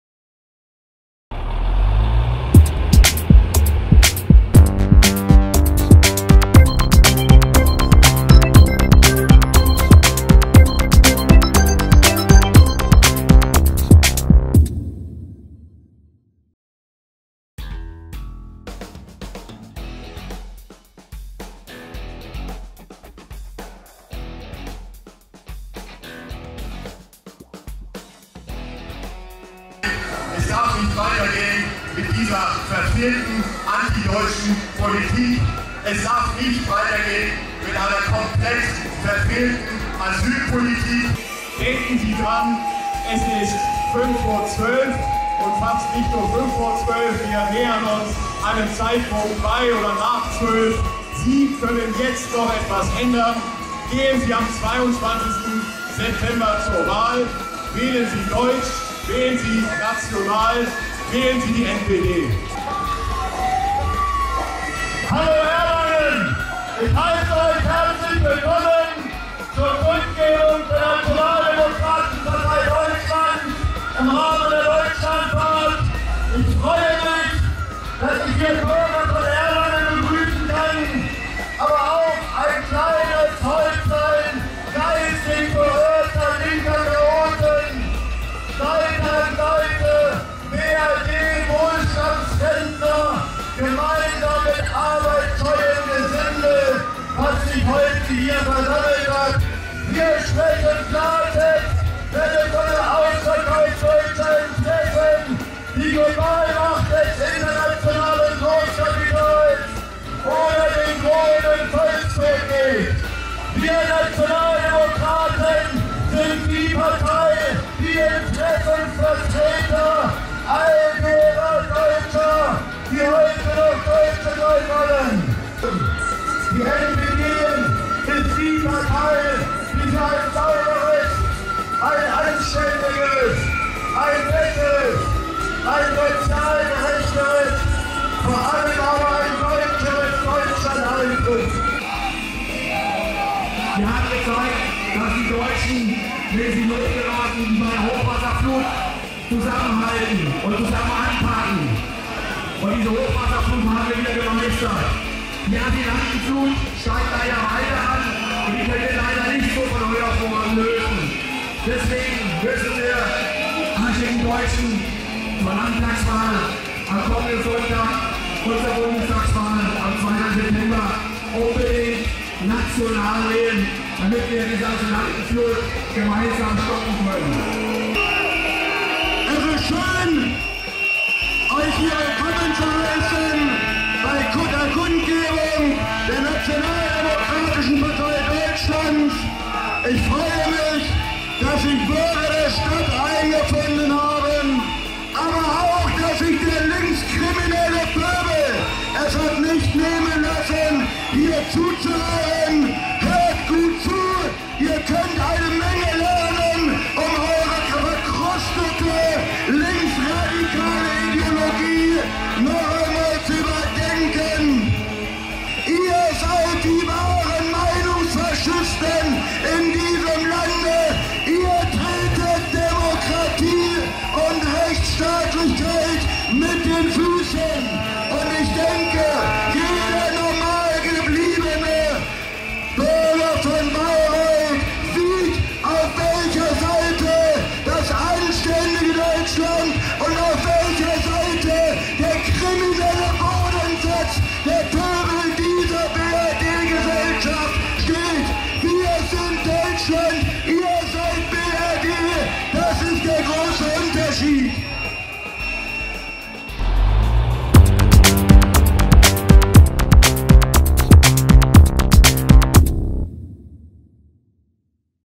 Deutschlandfahrt 2013 - Stationen in Erlangen und Bayreuth
Seit Samstag ist das Flaggschiff der NPD in Franken unterwegs. Erste Stationen am gestrigen Montag waren Erlangen und Bayreuth. Auch in den kommenden Tagen sind wir wieder für deutsche Interessen auf Achse!